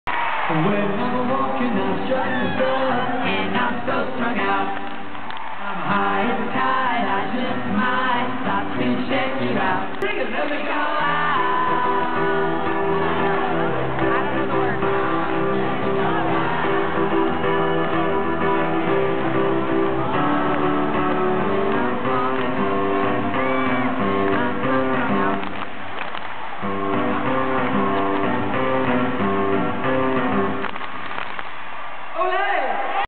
Keith right after he came out for the encore
Keith goofing around with the audience during the encore of his Little Rock concert December 2nd.